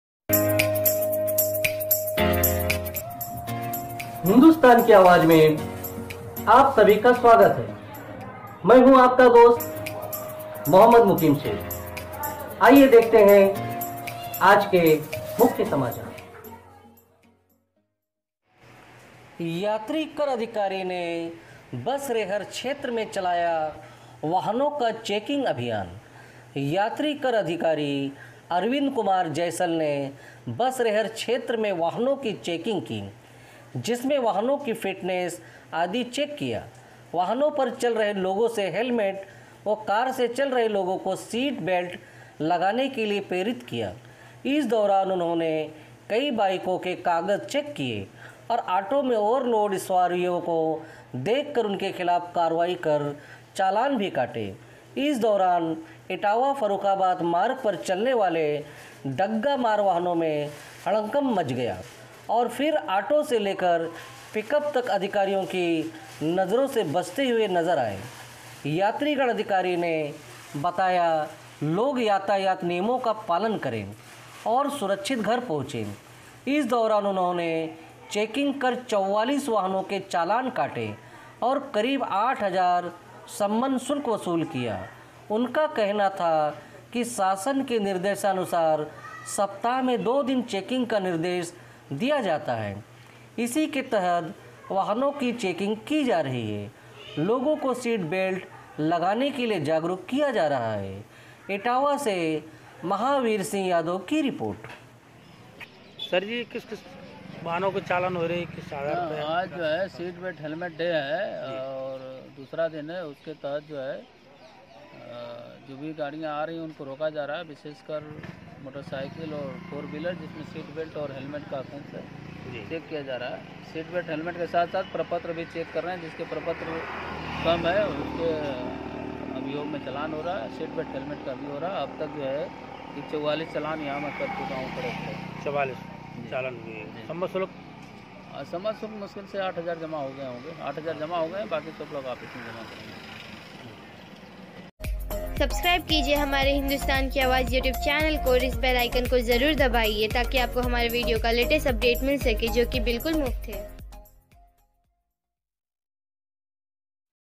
यात्रीकर अधिकारी ने बसरेहर क्षेत्र में चलाया वाहनों का चेकिंग अभियान । Hindustan Ki Aawaz
यात्रीकर अधिकारी ने बसरेहर  क्षेत्र में चलाया वाहनों का चेकिंग अभियान

यात्रीकर अधिकारी अरविंद कुमार जैसल ने बसरेहर क्षेत्र में वाहनों की चेकिंग की जिसमें वाहनों की फिटनेस आदि चेक किया बाइकों पर चल रहे लोगों से हेलमेट व कार से चल रहे लोगों को सीट बेल्ट लगाने के लिए प्रेरित किया इस दौरान उन्होंने कई बाइकों के कागज चेक किए और ऑटो में ओवरलोड सवारियों को देख कर उनके खिलाफ कार्रवाई कर चालान भी काटे इस दौरान इटावा फर्रुखाबाद मार्ग पर चलने वाले डग्गामार वाहनों में हड़कंप मच गया और ऑटो से लेकर पिकअप तक अधिकारियों की नजरों से बचते हुए नजर आए यात्रीगण अधिकारी ने बताया लोग यातायात नियमों का पालन करें और सुरक्षित घर पहुंचे इस दौरान उन्होंने चेकिंग कर 44 वाहनों के चालान काटे और करीब 8 हजार शमन शुल्क वसूल किया उनका कहना था कि शासन के निर्देशानुसार सप्ताह में 2 दिन चेकिंग का निर्देश दिया जाता है इसी के तहत वाहनों की चेकिंग की जा रही है लोगों को सीट बेल्ट लगाने के लिए जागरूक किया जा रहा है